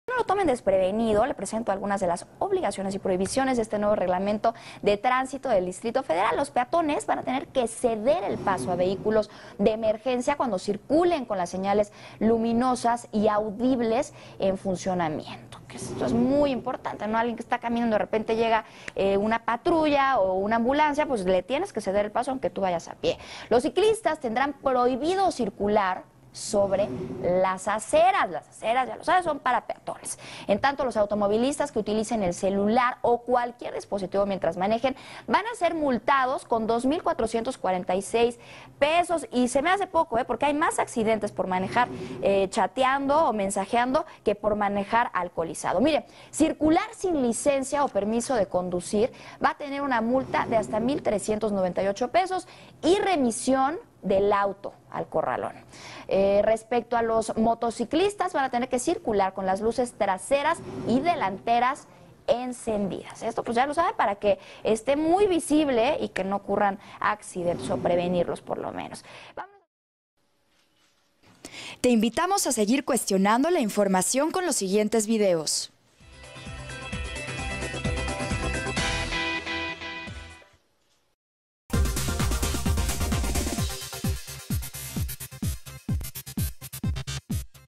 Obligaciones y prohibiciones del nuevo reglamento de tránsito del DF
Te presentamos las obligaciones más relevantes que destacó el nuevo reglamento de tránsito del Distrito Federal.

9 de septiembre 2015

Te invitamos a ver el siguiente video:

¿Qué opinan los mexicanos sobre el nuevo reglamento de tránsito?